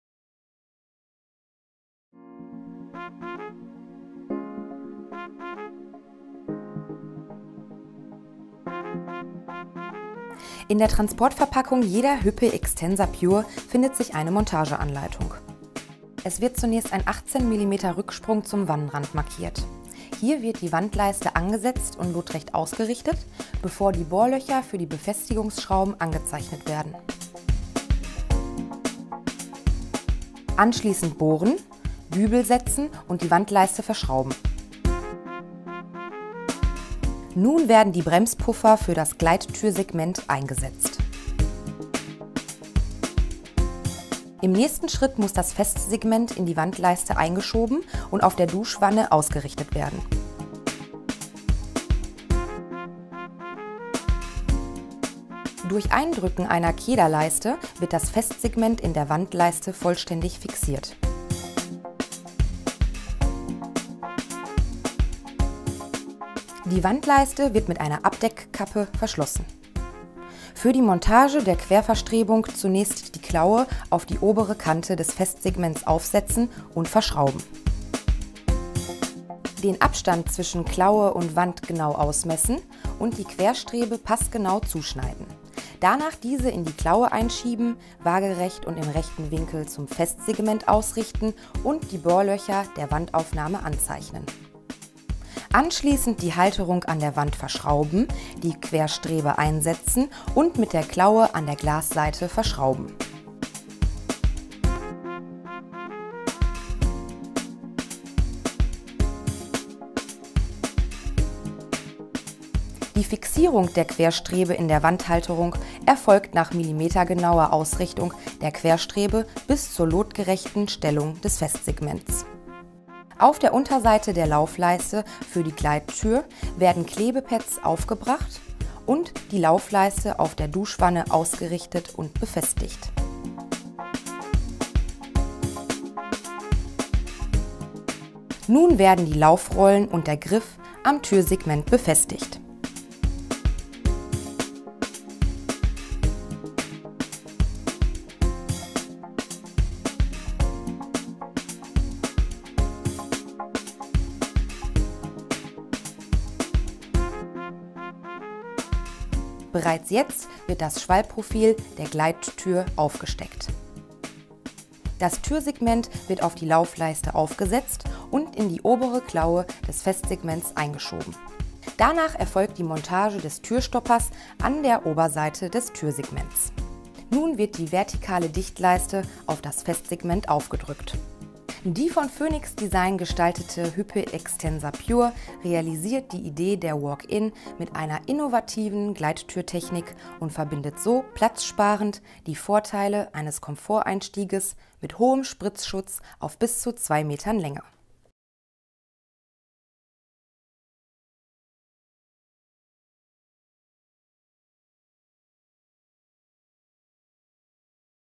HÜPPE Xtensa pure Montagevideo
In diesem Video demonstriert Ihnen Hüppe Aufbau und Montage einer Xtensa pure Duschabtrennung.